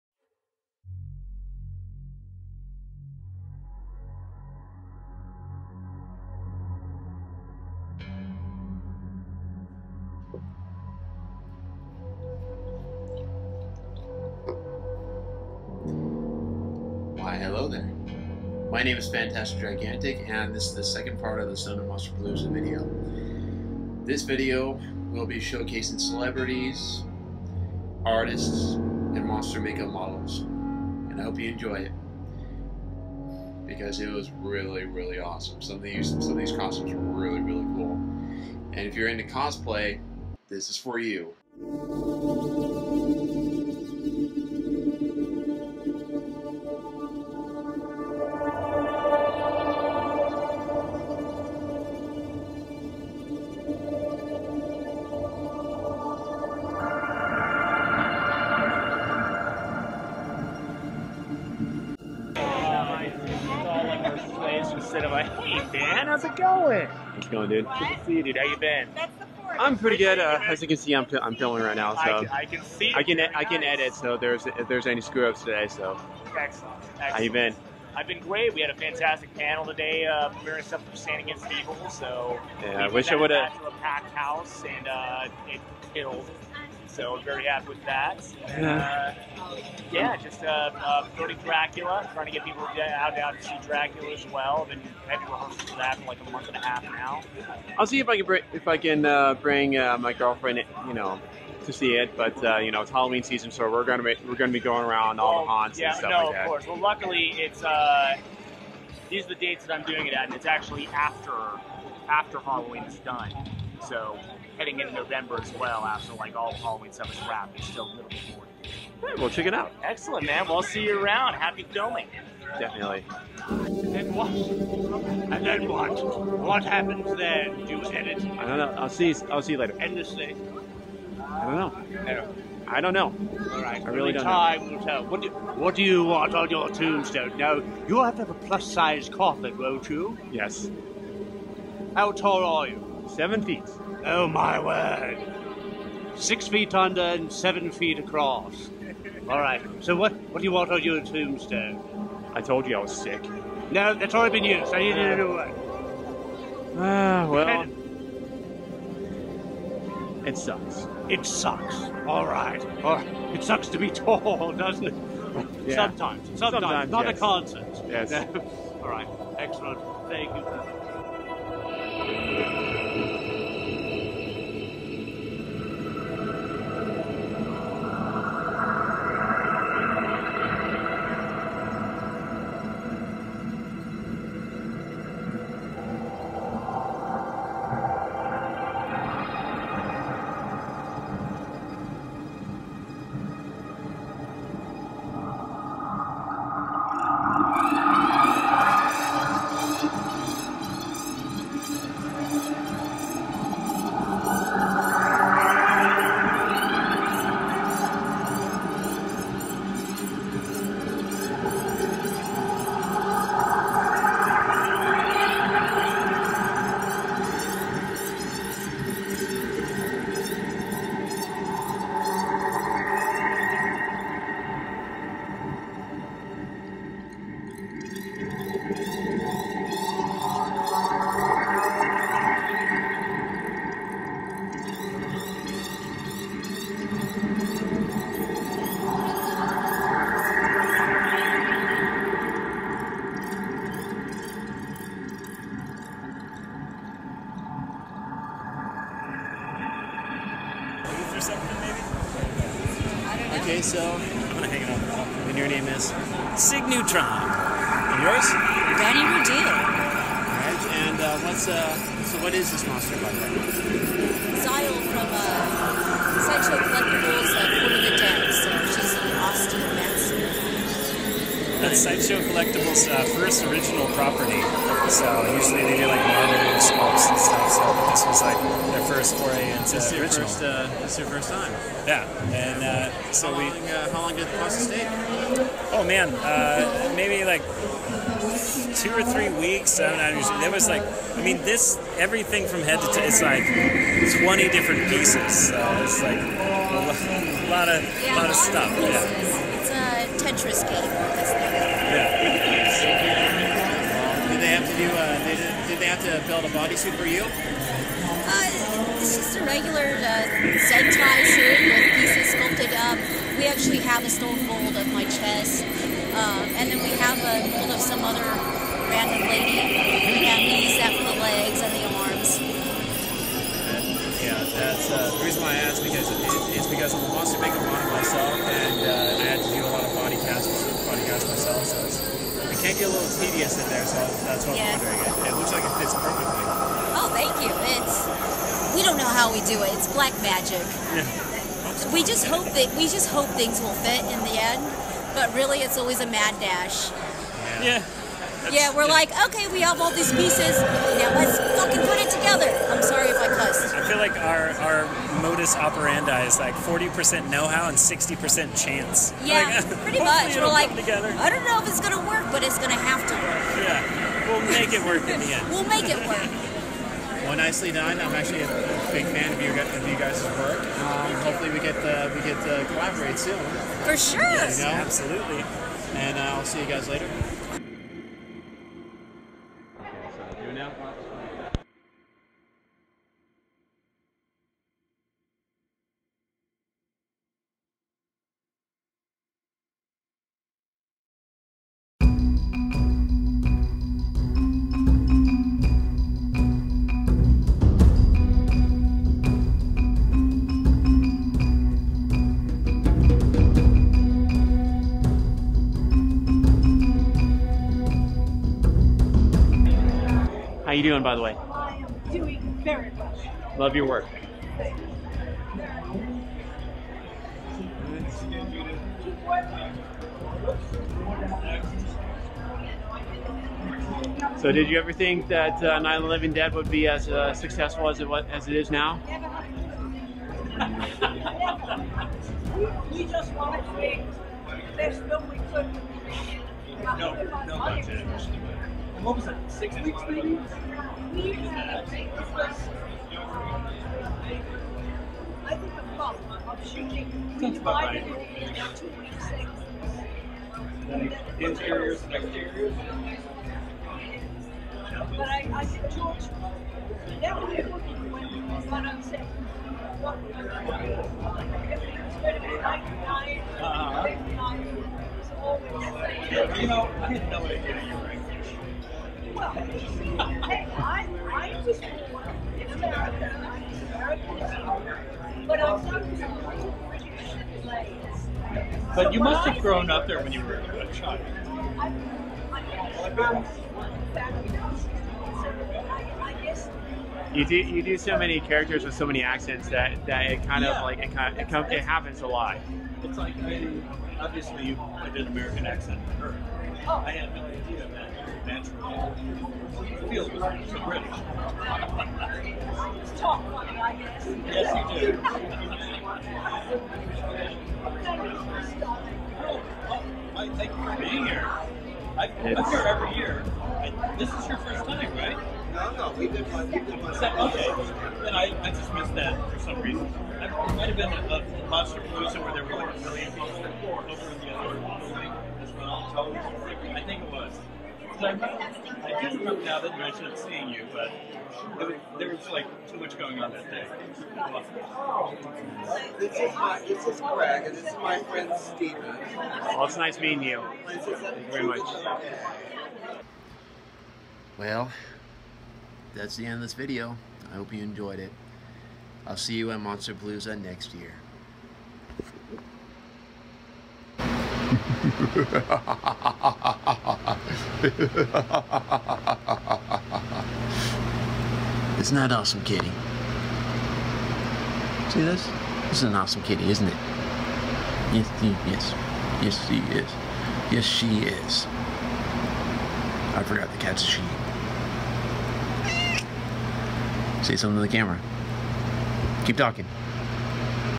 Son of Monsterpalooza 2016 Part 2
Here is the Second part of my Son of Monsterpalooza experience. This time the focus is on the Celebrity Wing, the Monster Makeup Models, and the artists behind the Monster Makeup Models.